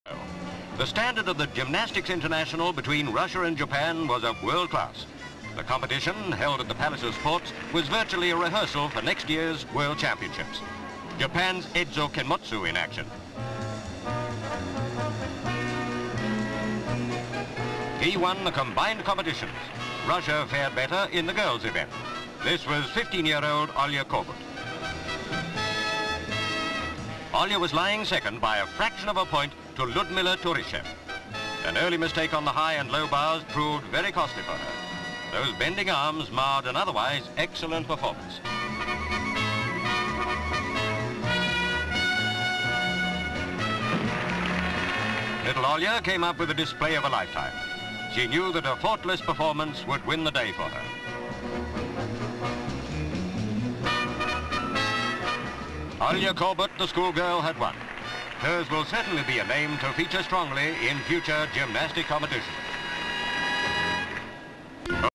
International Gymnastics (1969)
International gymnastics. Japan and Russia.

Moscow. MS Alexandra Maleyev, No. 11, swinging on rings, from Russia. LV Ditto, he comes to the end of his performance. SV Eidzu Kenmotsu of Japan on high bar. GV Ditto, he ends his performance. CU Olya Korbut, No. 14, from Russia, on the beam. GV Ditto, she does a complete somersault. GV Lyudmila Turischev, No. 16, from Russia, on high and low bars. She makes a mistake, she finishes her performance. CU Judge. SV No. 14, Olya Korbut on high and low bars, (the winner), ends performance. GV Crowd applauding. CU Young boys and girls with autograph books. SV Olya signing autographs.

Catalogue's note: The paperwork says Olya Korbut, though possibly Olga.
 FILM ID:2237.03